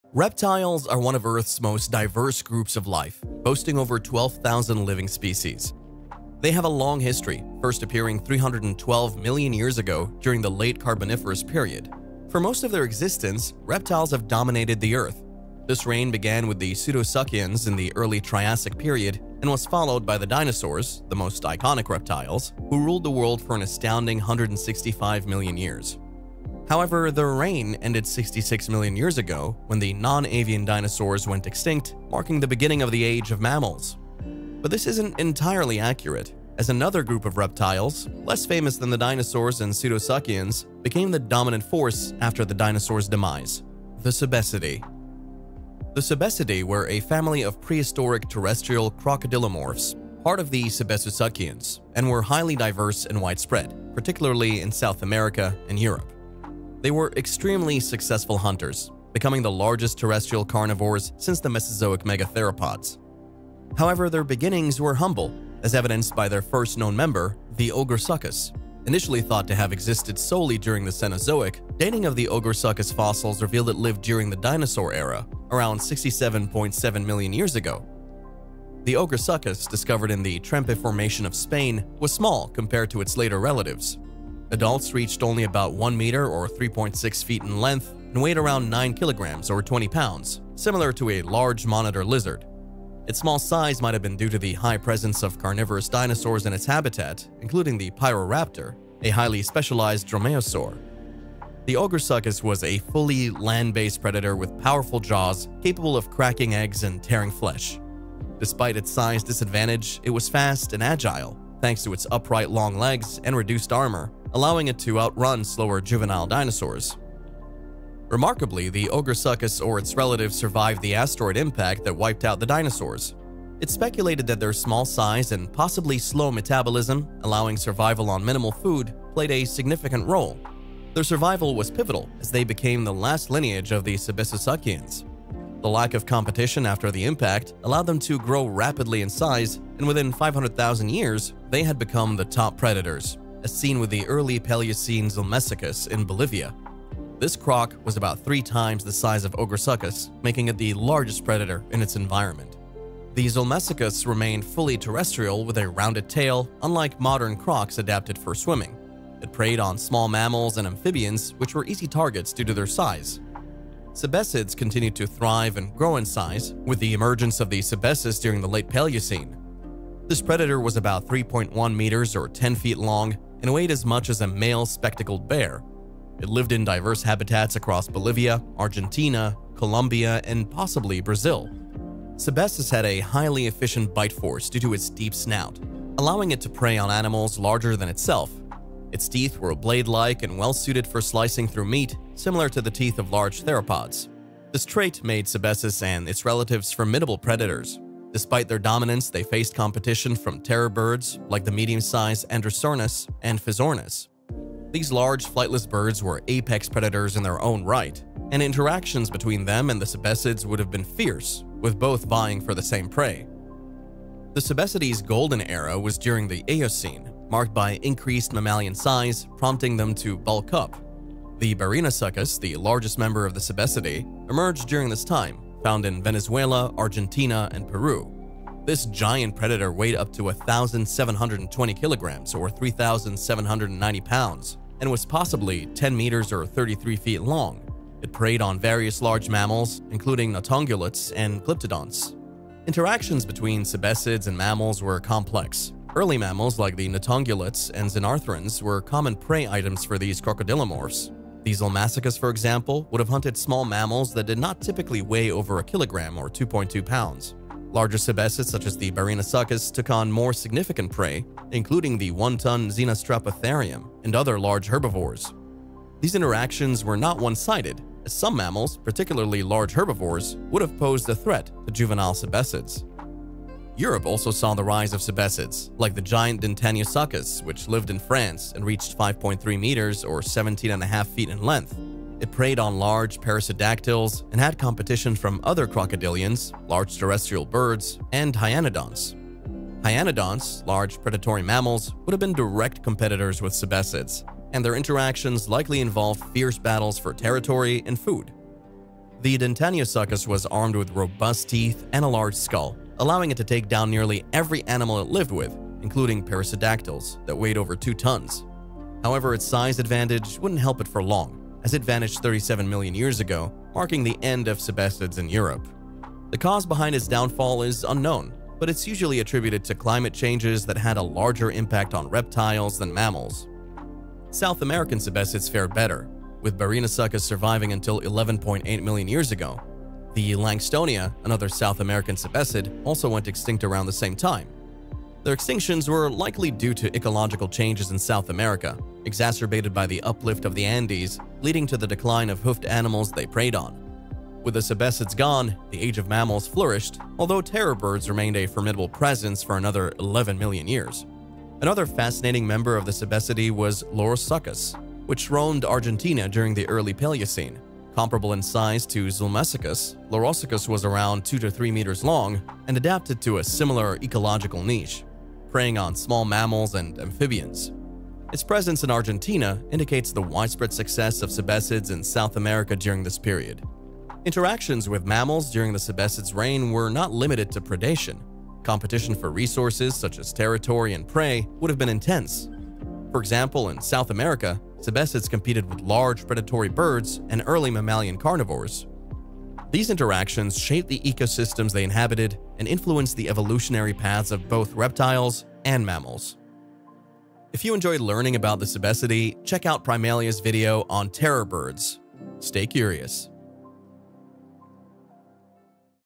Dinosaur Extinction Survivor: The Reign of the Sebecidae
Welcome to Primalia! 🌍

Explore the fascinating world of the Sebecidae, a family of prehistoric crocodile-like reptiles that once dominated South America and elsewhere. In this exciting episode, we delve into the rise and fall of these fearsome predators, from their humble beginnings to their reign as top land carnivores during the Cenozoic era. Learn about the incredible adaptations that made the Sebecidae such efficient hunters, their diverse range of species, and the unique environments they thrived in.

These were the largest land carnivores since the dinosaurs, and they had an impressive 30-million-year reign. We'll be sure to discuss interactions with another rising power in the animal kingdom - mammals! As well as other creatures that shared their world.

Join us as we uncover the secrets of these ancient giants and explore their impact on prehistoric ecosystems.